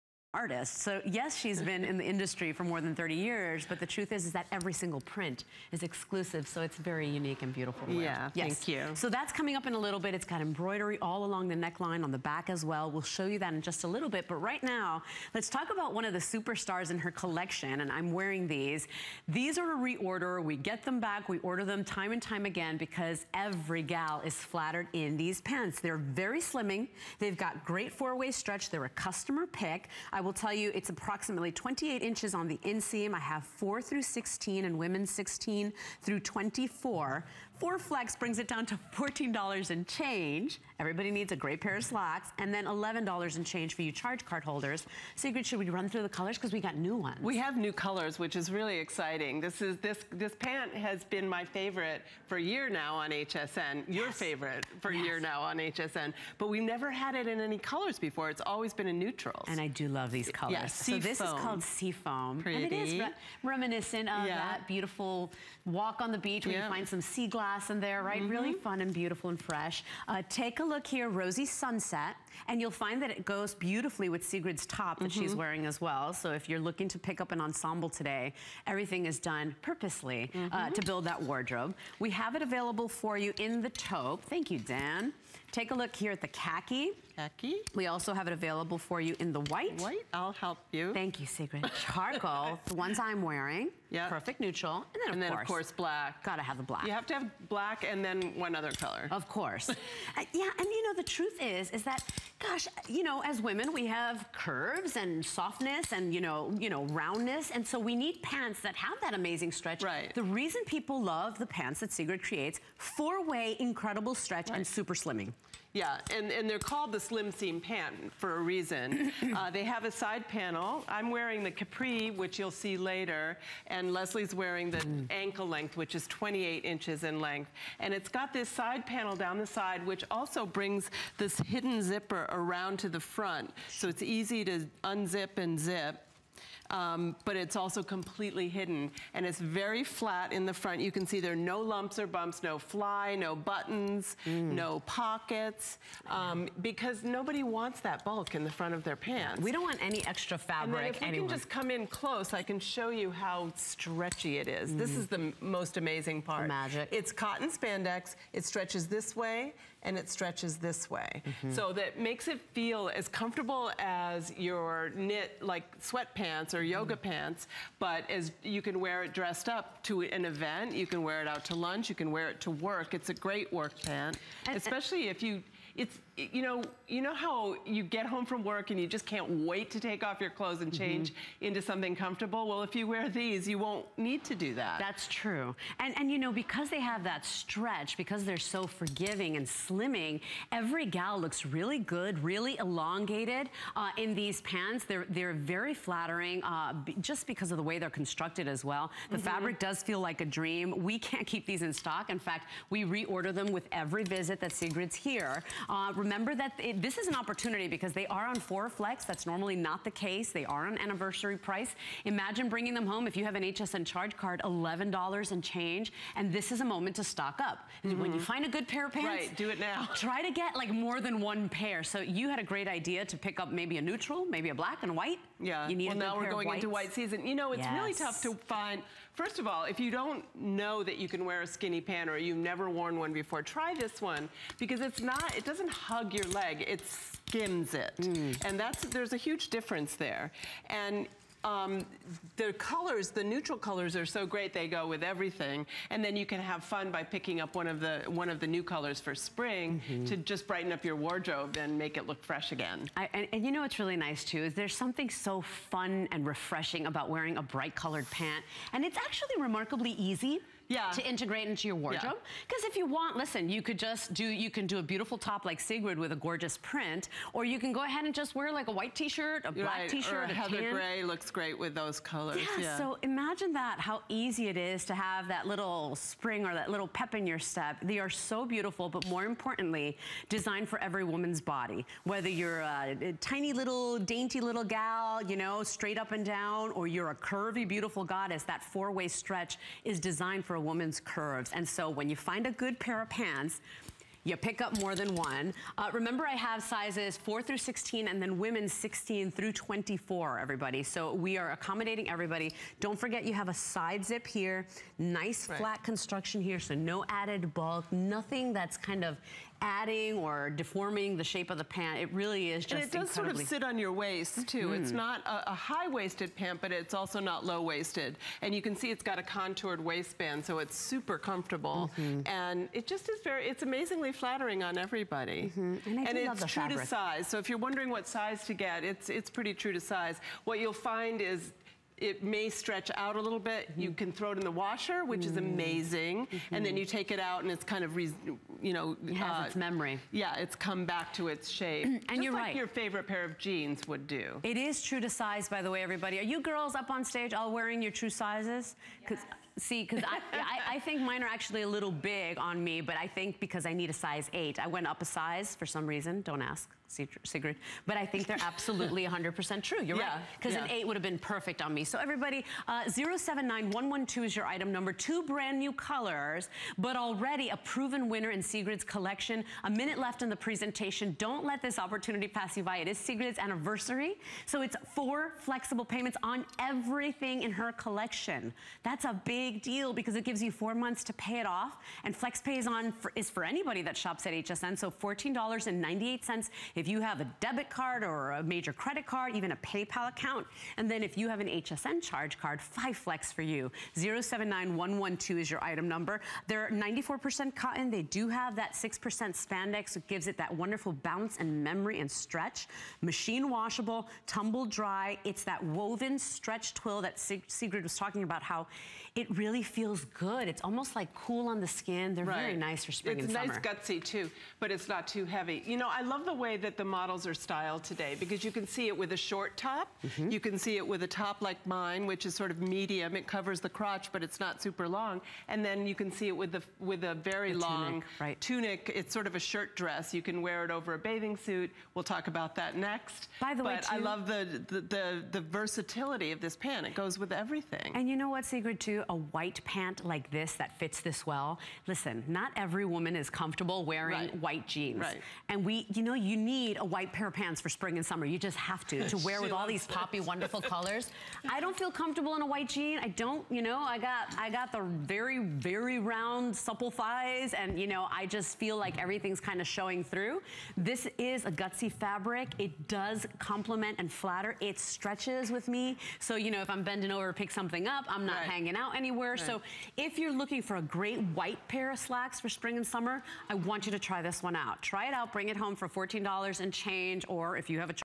Sigrid Olsen Studio BiStretch Slim Seam Pant
Slim pants are the foundation for any chic wardrobe. So versatile, so modern. You'll wear them with everything from flat sandals to strappy heels...